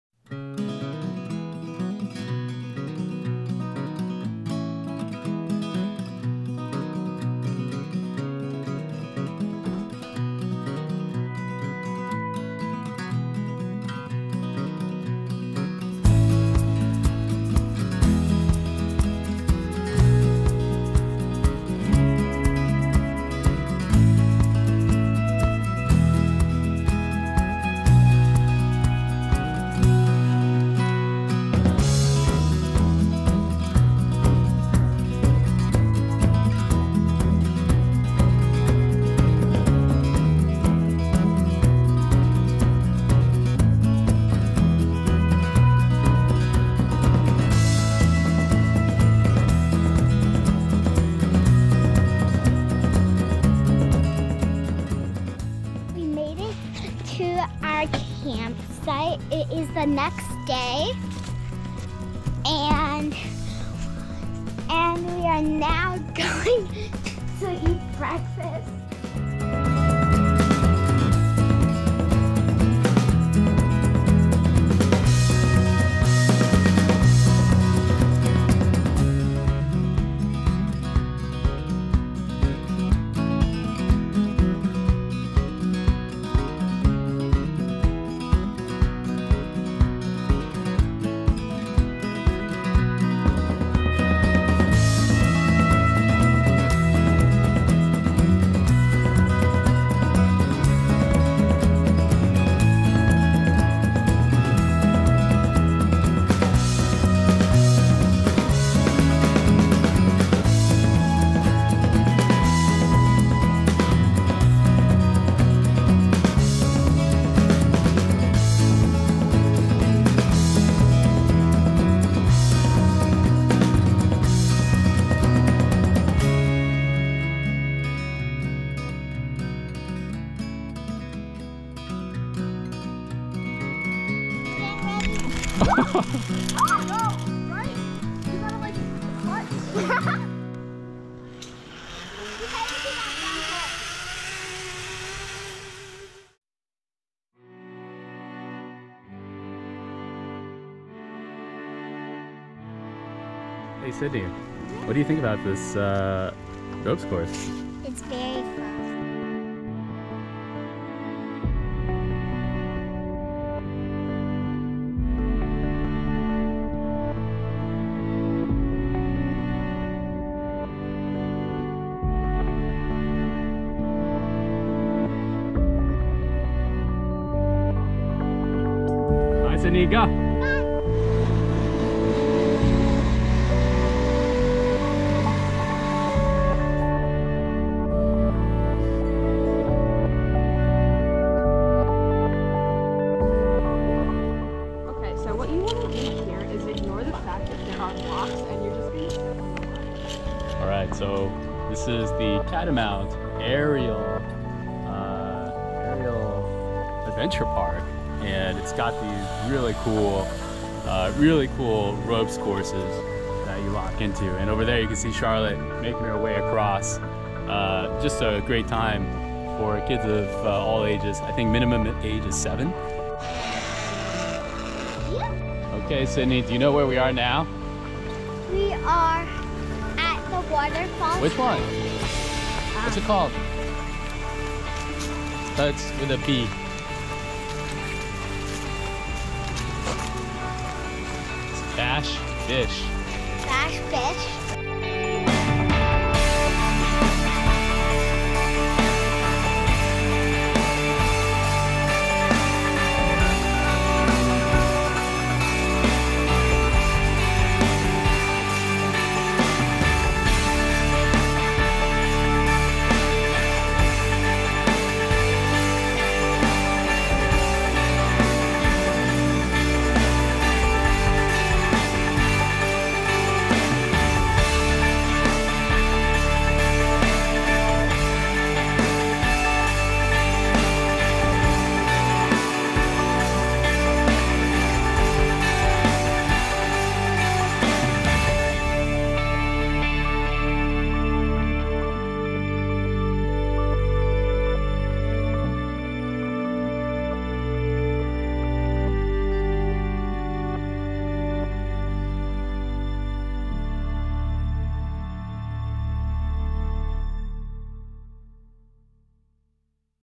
Copake, NY - Catamount Aerial Adventure Park - Bash Bish Falls - October, 2023
A weekend trip up to Copake, NY with an unbelievable day at Catamount Aerial Adventure Park in a carpet of golden leaves!  Then a hike to the striking Bash Bish Falls which takes you across the NY/MA border along the way